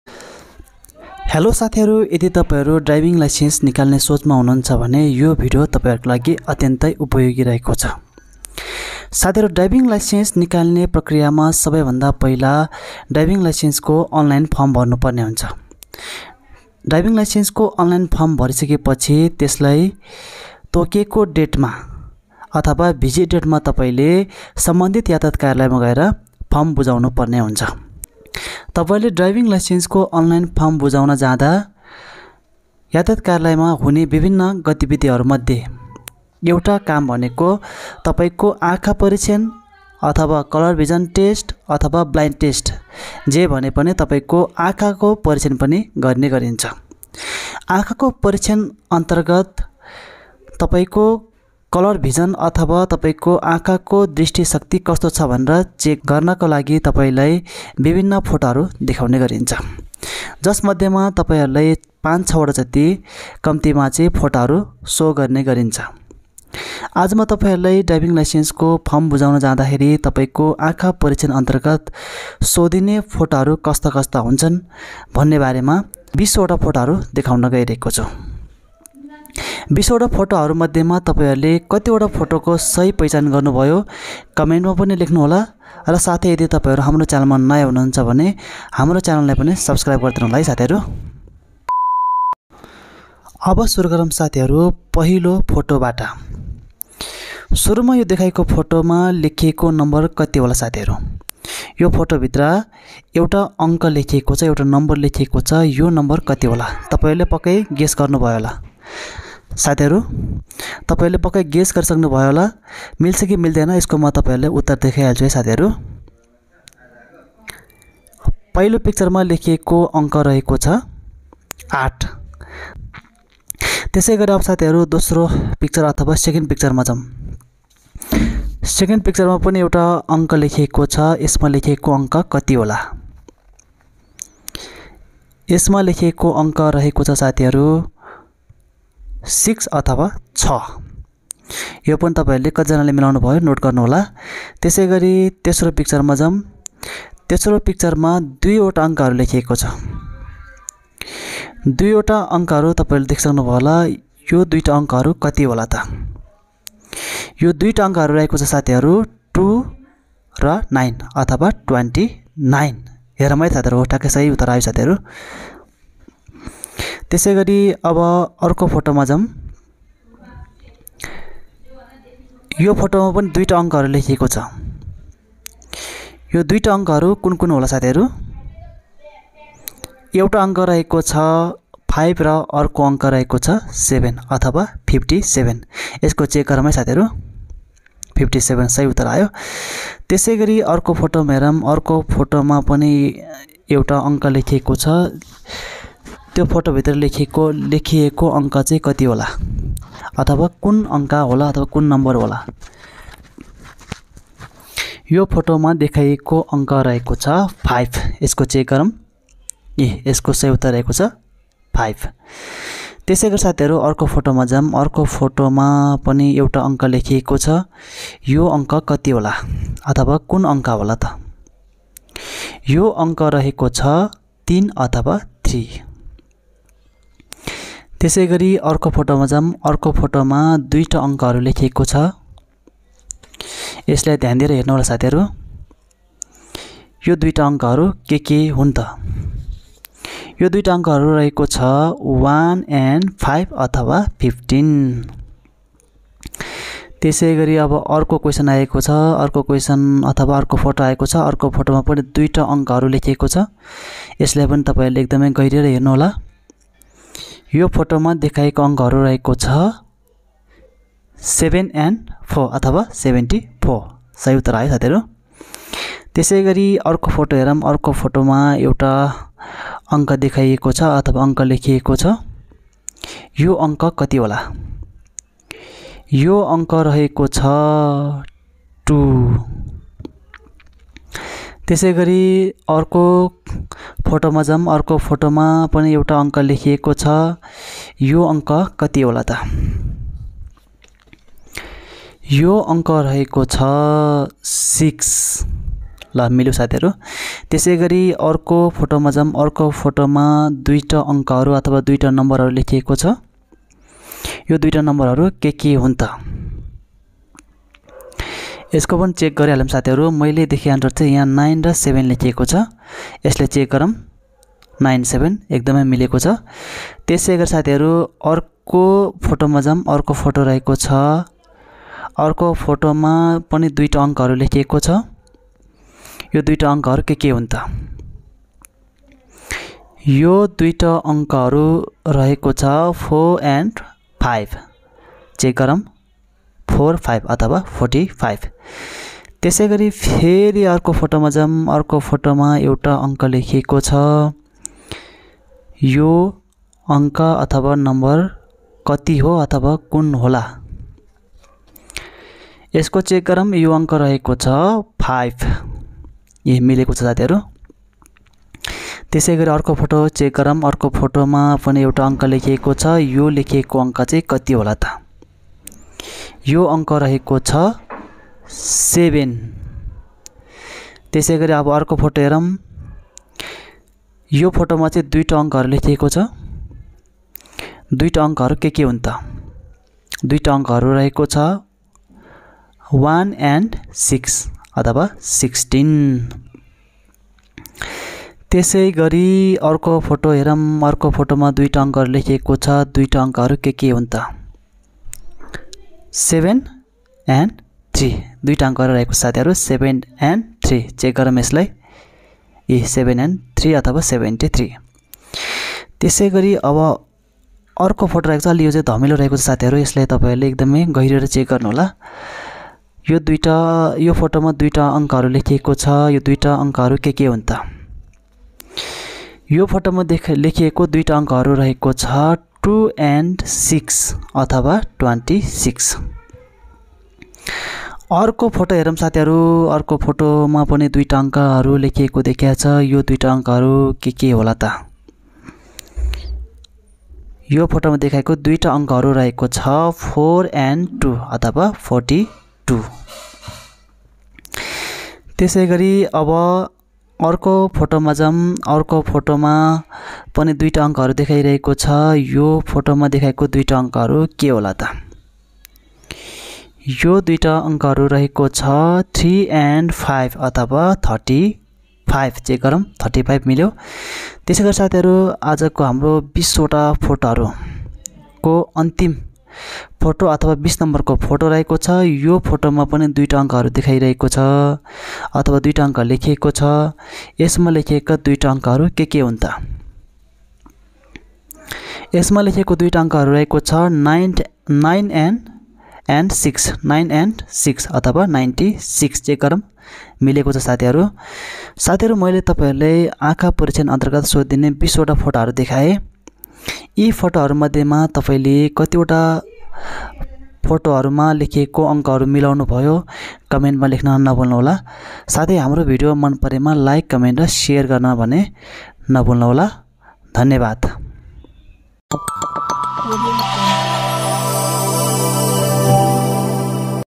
Color Vision Test for Nepali Driving License | Blind Test/Eye Test for Driving Licence in Nepal
Color Vision Test Photos for Nepali Driving License | Blind Test/Eye Test for Licence in Nepal |

Hi friends,maile yas videoma Nepalma driving license ko akha checkingma kasto khalko photo haru dekhauxan. Soko information garako xu. Driving license ko eye checkingma videoma dekhaye jastai picture haru dekhauxan. Asha xa sathiharu video lai pura herera tapaiharule afno akhako dristi shakti kasto xa vanera thaha paunexan. Keep supporting me,thanks all.